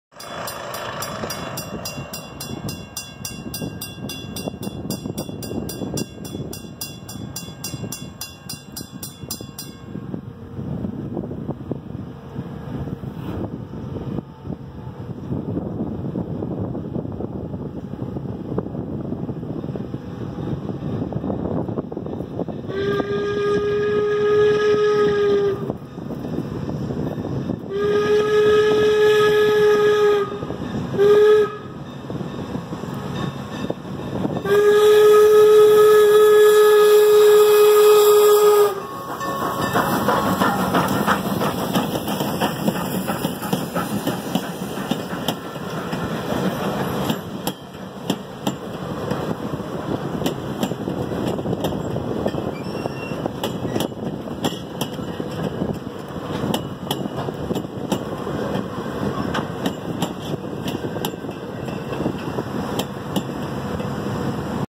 N&W 475 at Carpenter's Crossing
After running around her train at Paradise, N&W 475 heads back to town at Carpenter's Crossing.